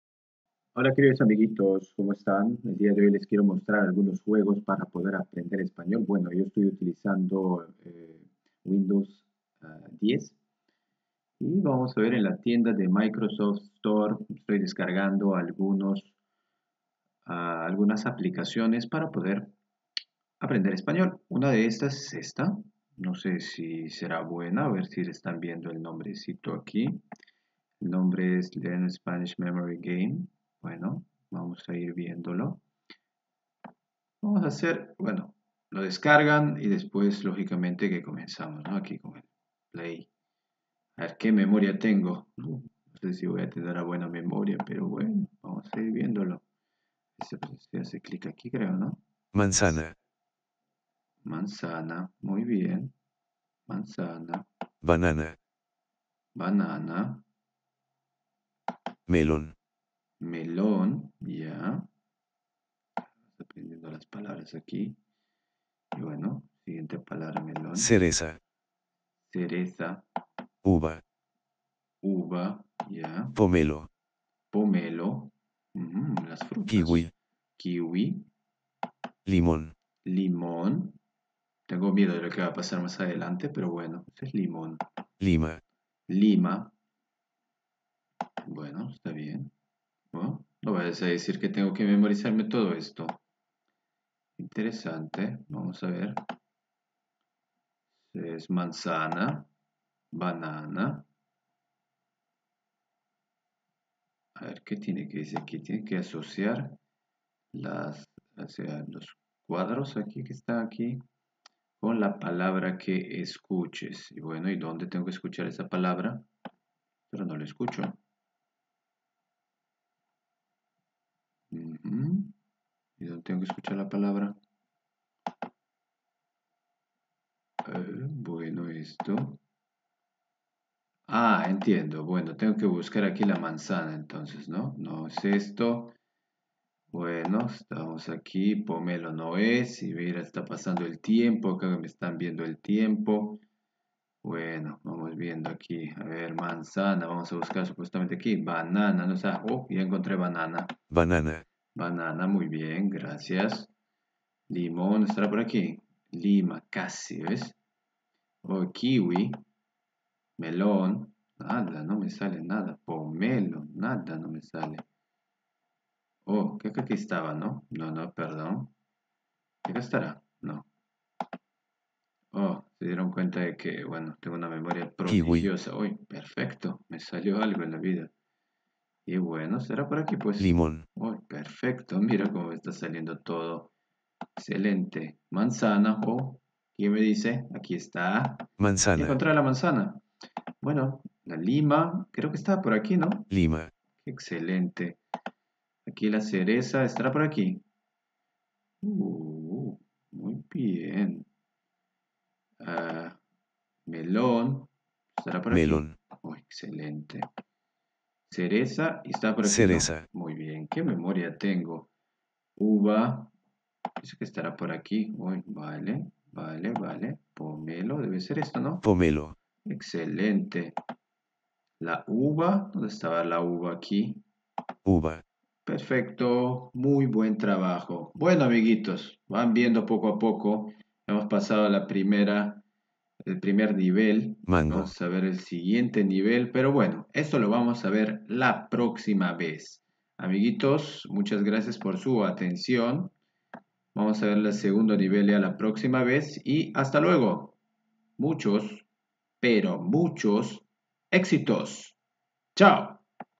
JUGANDO EN ESPAÑOL(FRUTAS)
Hola queridos amigos!
Aprendemos el español con ayuda de juegos.
¡Diviértanse!